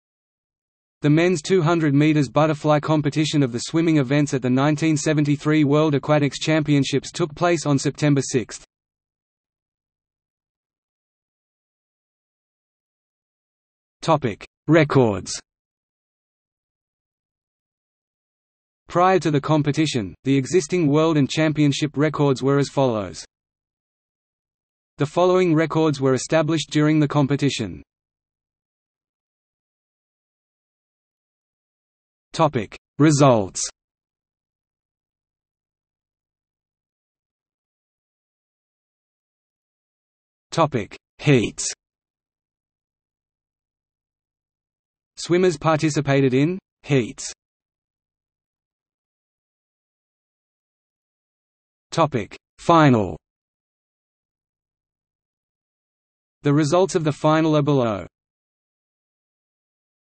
Swimming at the 1973 World Aquatics Championships – Men's 200 metre butterfly | Wikipedia audi ...
00:00:10 1 Records
00:00:28 2 Results
00:00:37 2.1 Heats
00:00:48 2.2 Final

- Socrates

SUMMARY
The men's 200 metre butterfly competition of the swimming events at the 1973 World Aquatics Championships took place on September 6.